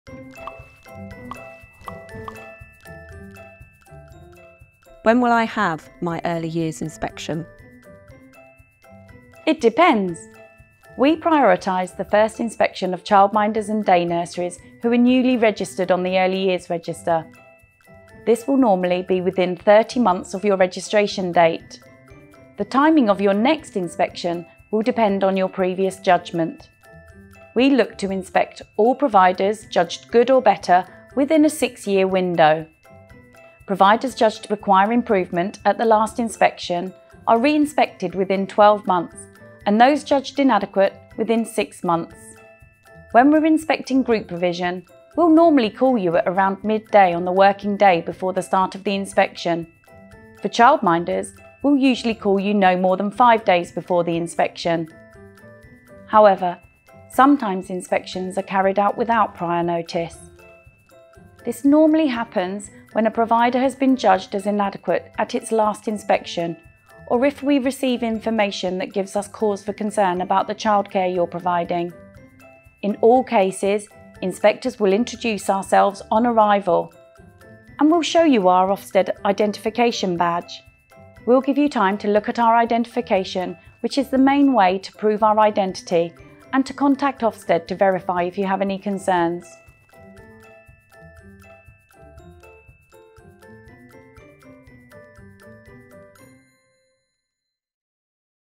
When will I have my early years inspection? | Early years FAQs
Our Early Years Regulatory Inspector and Early Education Quality and Practice Lead Sam Sleeman-Boss answers one of the most common questions we receive: ‘When will I have my early years inspection?’. She explains the number of factors that are considered when early years settings are selected for inspection.

You can find more information in our early years inspection handbook